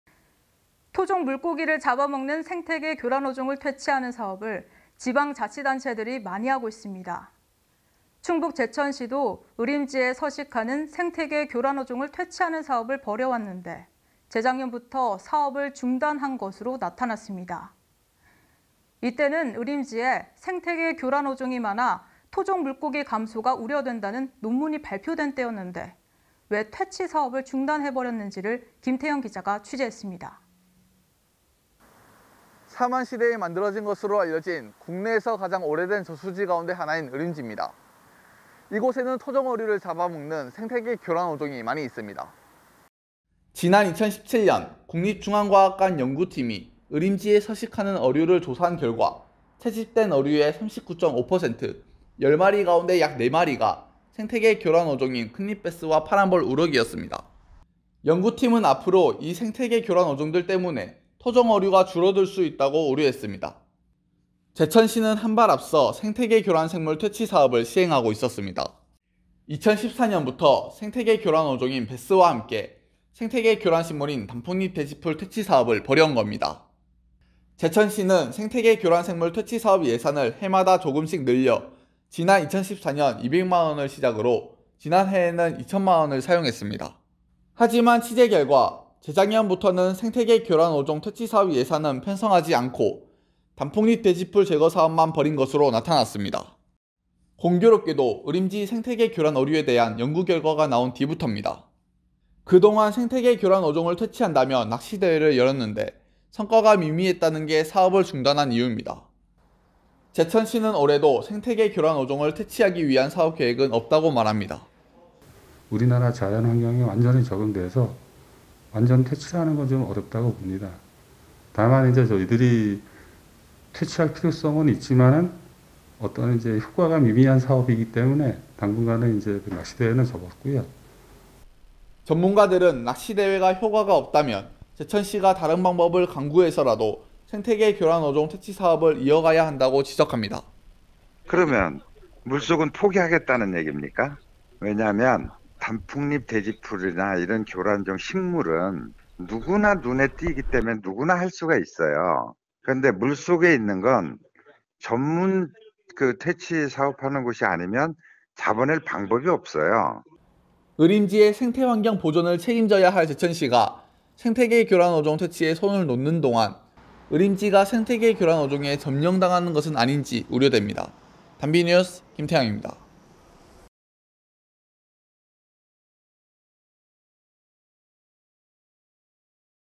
[단비리포트] ‘낚시대회’ 성과 적다고 퇴치사업 접어
[앵커]
토종 물고기를 잡아먹는 생태계 교란 어종을 퇴치하는 사업을 지방자치단체들이 많이 하고 있습니다.

충북 제천시도 의림지에 서식하는 생태계 교란 어종을 퇴치하는 사업을 벌여왔는데, 재작년부터 사업을 중단한 것으로 나타났습니다.

이때는 의림지에 생태계 교란 어종이 많아 토종 물고기 감소가 우려된다는 논문이 발표된 때였는데, 왜 퇴치 사업을 중단해버렸는지, 김태형 기자가 취재했습니다.

[리포트]
삼한시대에 만들어진 것으로 알려진, 국내에서 가장 오래된 저수지 가운데 하나인 의림지입니다.

이곳에는 토종어류를 잡아먹는 생태계 교란어종이 많이 있습니다.

지난 2017년, 국립중앙과학관 연구팀이 의림지에 서식하는 어류를 조사한 결과, 채집된 어류의 39.5%, 10마리 가운데 약 4마리가 생태계 교란 어종인 큰입배스와 파랑볼우럭이었습니다.

연구팀은 앞으로 이 생태계 교란 어종들 때문에 토종 어류가 줄어들 수 있다고 우려했습니다.

제천시는 한발 앞서 생태계 교란 생물 퇴치사업을 시행하고 있었습니다.

2014년부터 생태계 교란 어종인 배스와 함께 생태계 교란 식물인 단풍잎돼지풀 퇴치 사업을 벌여온 겁니다.

제천시는 생태계 교란 생물 퇴치 사업 예산을 해마다 조금씩 늘려, 지난 2014년 200만 원을 시작으로 지난해에는 이천만 원을 사용했습니다.

하지만 취재결과 재작년부터는 생태계 교란 어종 퇴치사업 예산은 편성하지 않고 단풍잎돼지풀 제거사업만 벌인 것으로 나타났습니다.

공교롭게도 의림지 생태계 교란 어류에 대한 연구 결과가 나온 뒤부터입니다.

그동안 생태계 교란 어종을 퇴치한다며 낚시 대회를 열었는데 성과가 미미했다는 게 사업을 중단한 이유입니다.

제천시는 올해도 생태계 교란 어종을 퇴치하기 위한 사업 계획은 없다고 말합니다.

[원철규 / 제천시 자연환경과 주무관 : (배스와 파란볼우럭은) 우리나라 환경에 완전히 적응돼서 완전 퇴치라는 것은 어렵다고 봅니다. 다만, 저희들이 퇴치할 필요성은 있지만 효과가 미미한 사업이기 때문에 당분간 낚시대회는 접었고요.]

전문가들은 낚시 대회가 효과가 없다면 제천시가 다른 방법을 강구해서라도 생태계 교란 어종 퇴치 사업을 이어가야 한다고 지적합니다.

[한신철 / 한국생태계교란어종 퇴치관리협회장 : 그러면 물 속은 포기하겠다는 얘깁니까? 왜냐하면 단풍잎교란종 같은 식물은 누구나 눈에 띄기 때문에 누구나 (제거)할 수가 있어요. 그런데 물 속에 있는 건 전문 퇴치 사업하는 곳이 아니면 잡아낼 방법이 없어요.]

의림지의 생태환경 보전을 책임져야 할 제천시가 생태계 교란 어종 퇴치에 손을 놓는 동안 의림지가 생태계 교란 어종에 점령당하는 것은 아닌지 우려됩니다.

단비뉴스 김태형입니다.

(CG : 신현우 / 편집 : 김태형 / 촬영 : 이동민, 김태형 / 앵커 : 정진명)